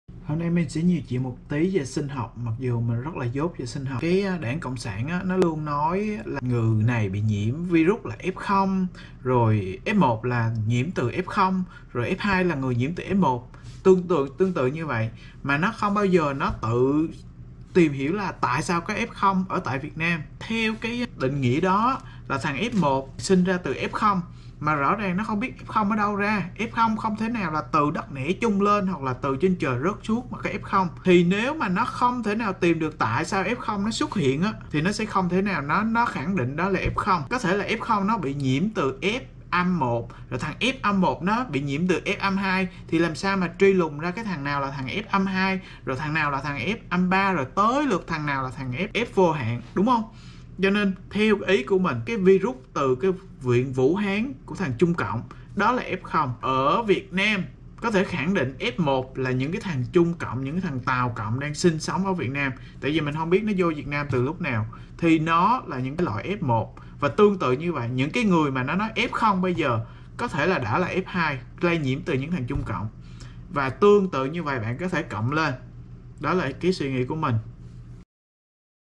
Chính Trị - F0 ở trên trời xuống hay từ đất nẻ chui lên? Binh Dang
Nếu như F0 bị nhiễm từ F âm 1, F âm 1 nhiễm từ F âm 2. Vậy cách ly F0 chẳng khác nào chặn cái ngọn.
Đó là cách chống dịch dốt của đảng cộng sản.